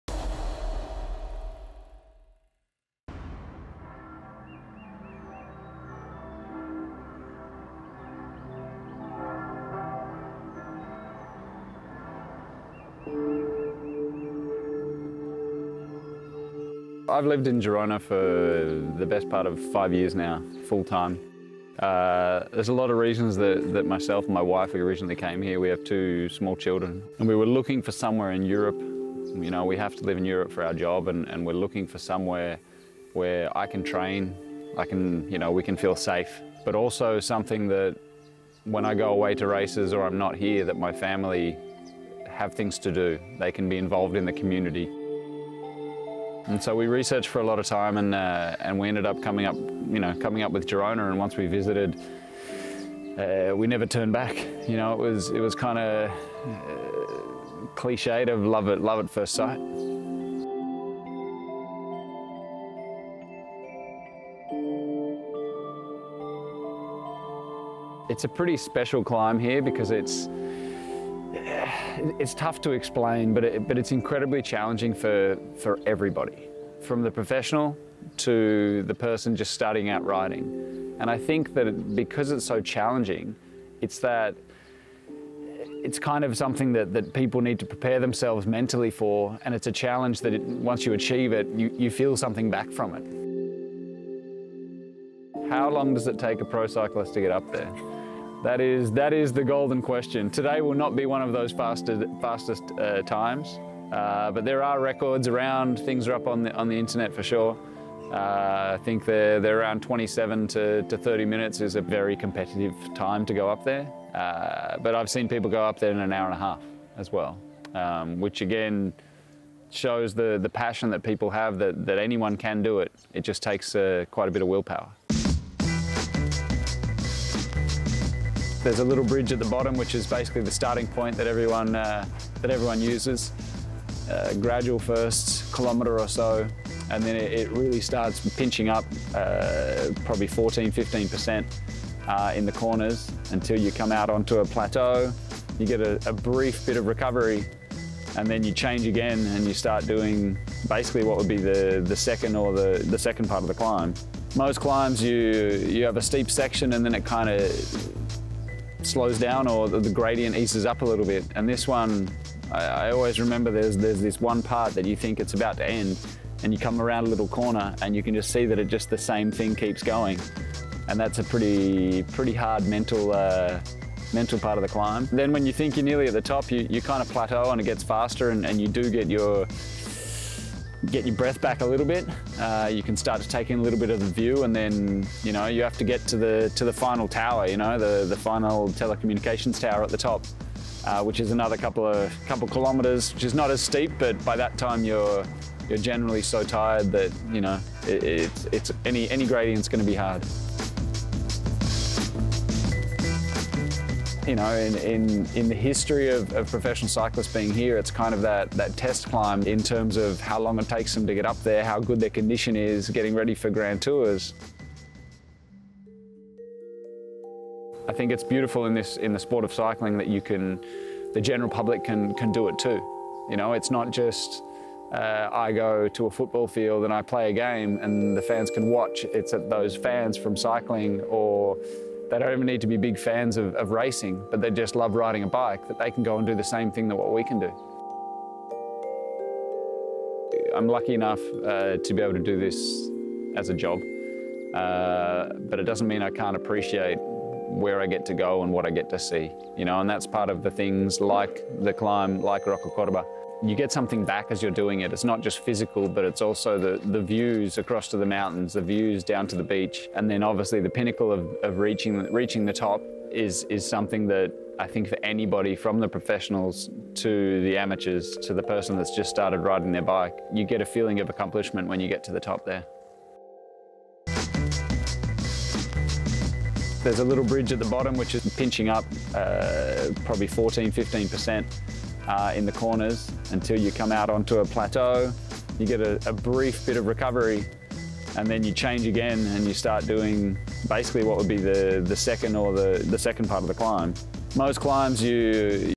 Girona: a Cyclists Destination of Choice | inCycle
UAE Team Emirates rider Rory Sutherland tells us why Girona is his home and cycling destination of choice.